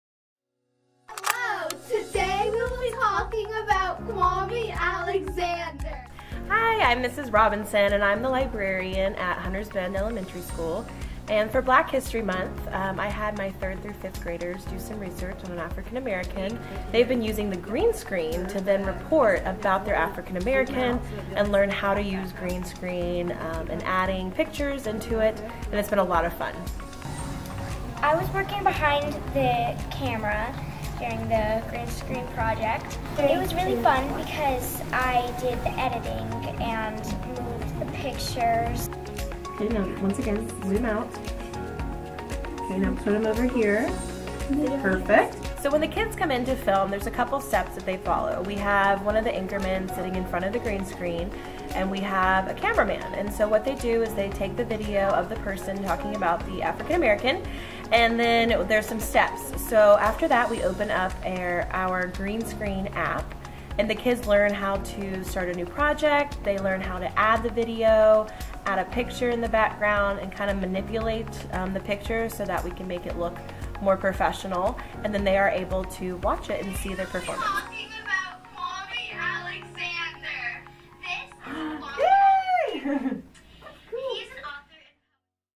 HBES Students Create Videos Celebrating African Americans
As part of Black History Month, students at Hunters Bend Elementary have been creating and sharing presentations that honor and celebrate the contributions of African Americans. Thank you to HBES Good News Ambassador Shannon Albee for providing this footage.